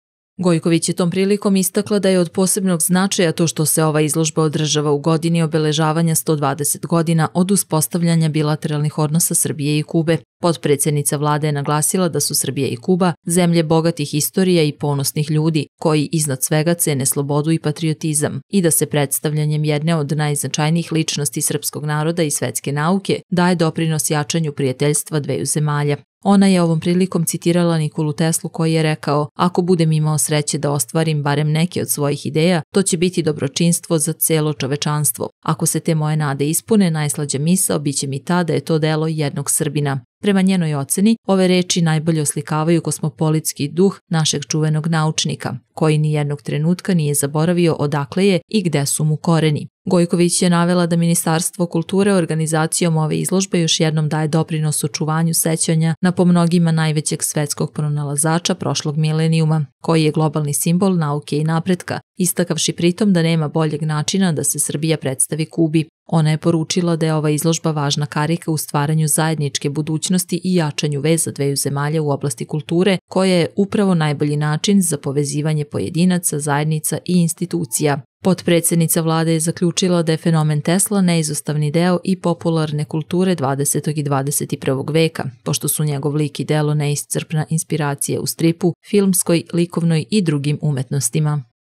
Gojković u Havani otvorila izložbu „Energija za budućnost – Nikola Tesla, razvoj hidroenergetike”
Potpredsednica Vlade Republike Srbije i ministarka kulture Maja Gojković otvorila je u Havani izložbu „Energija za budućnost – Nikola Tesla, razvoj hidroenergetike”, posvećenu jednom od najznačajnijih srpskih i svetskih naučnika.